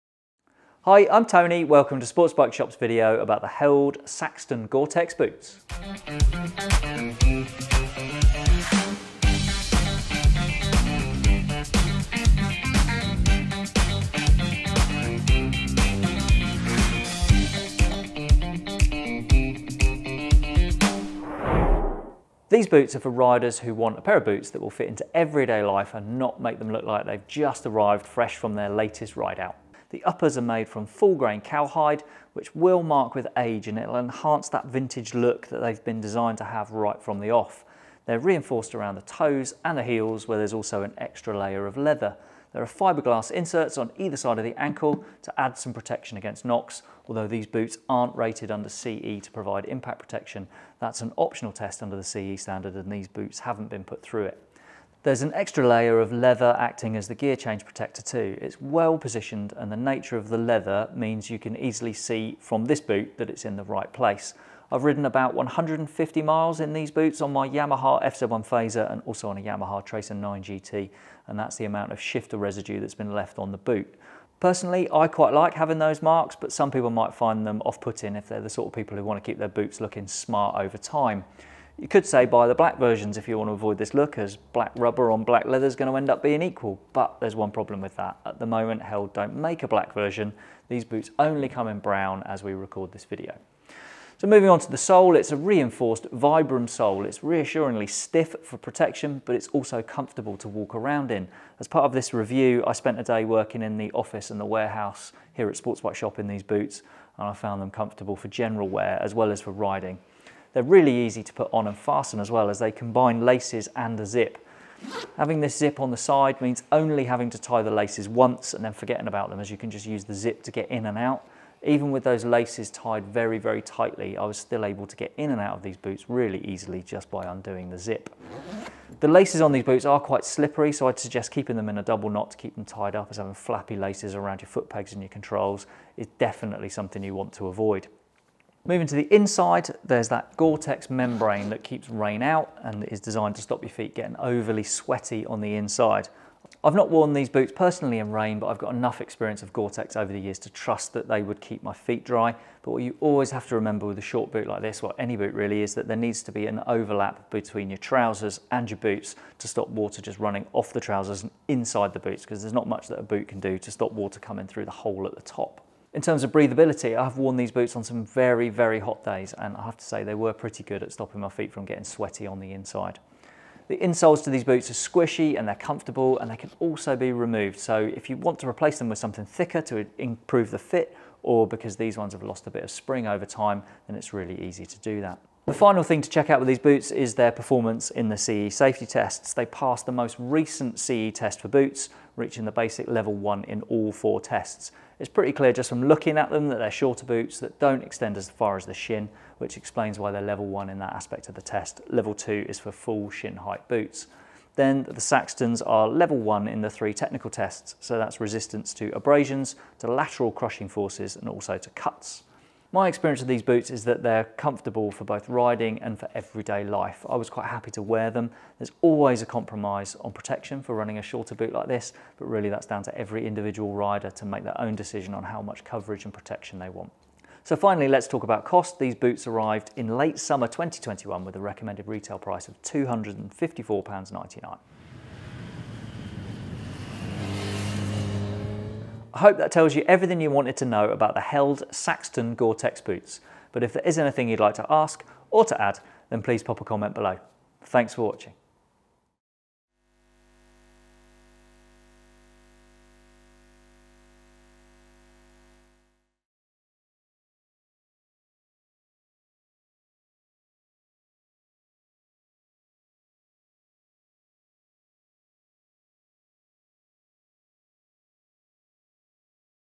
Held Saxton Gore-Tex motorcycle boots review - Sportsbikeshop
The Held Saxton boots land in 2021 to bring another subtle option for riders who want boots that blend easily into everyday life. Throw in a Gore-Tex membrane and there’s plenty of appeal.